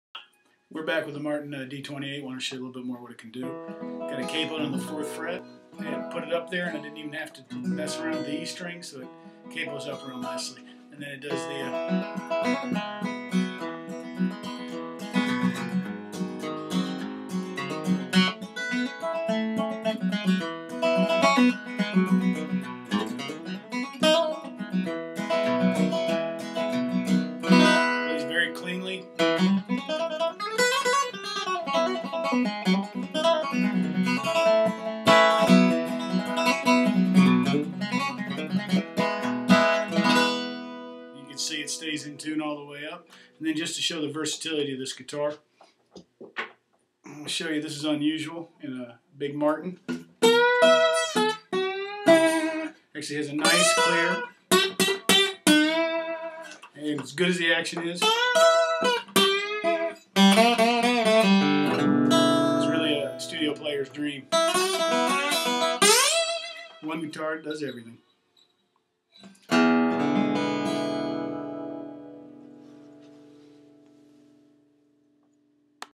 1968 Martin D-28 Brazilian Rosewood TAKE 2 (in the studio)
Take 2 in the studio.

This is a gorgeous D-28 soon to be for sale on ebay. I will update the link when the auction is live. This was recorded in the store using a USB Audio Technica microphone directly into iMovie. It isn't the greatest quality video but we wanted to show the guitar in use.